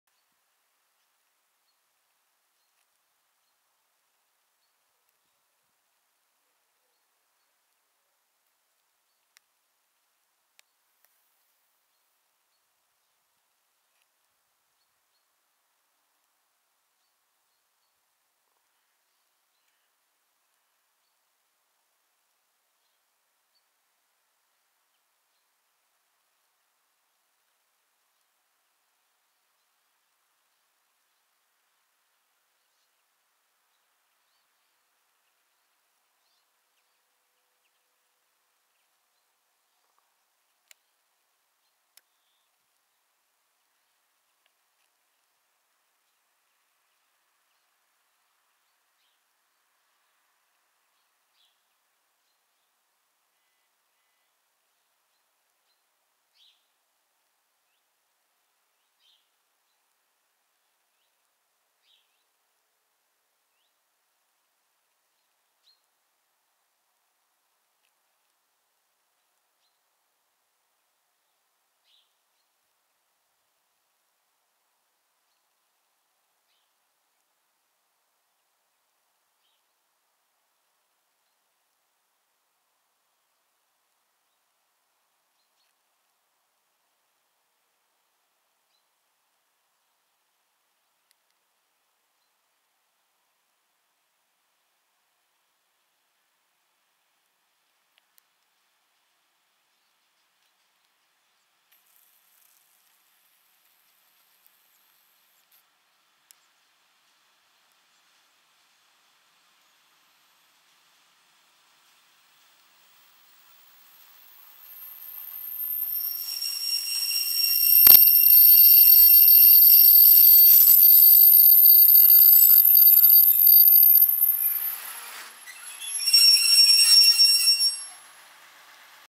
Gleisbauzug in Reußen
einfahrt eines Gleisbauzuges in den Bahnhof Reußen